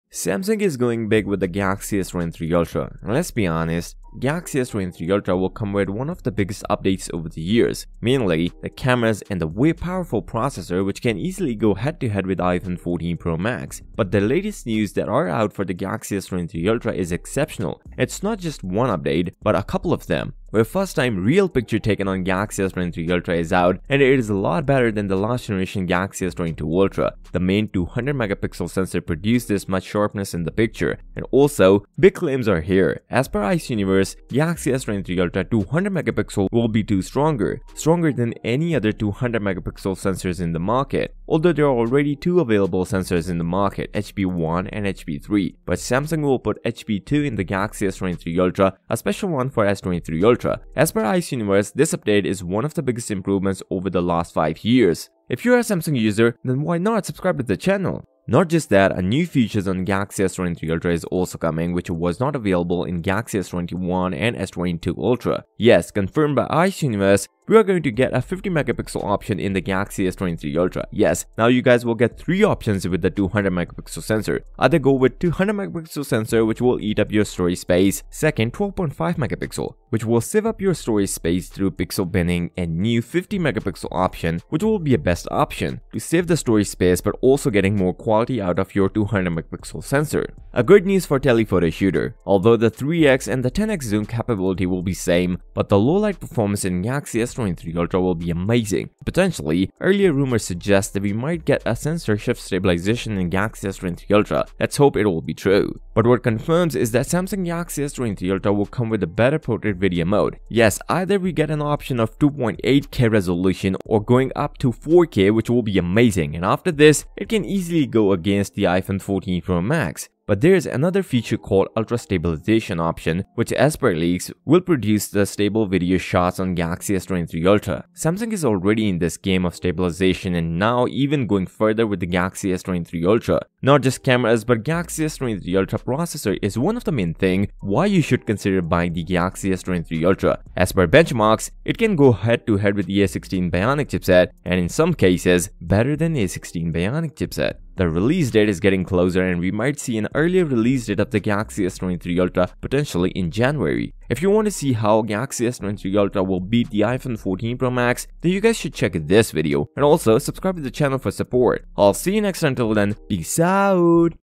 Samsung Galaxy S23 Ultra - First 200MP CAMERA TEST SHOT
Hello everyone in this video I am going to talk about topics included:

Samsung Galaxy S23 Ultra - The first ever camera test of Galaxy S23 Ultra is out.

Samsung Galaxy S23 Ultra - Galaxy S23 Ultra comes with surprising new features and way better sharpness than its predecessor. For Android users Galaxy S23 Ultra could be one of the best device for next year.

By DKHD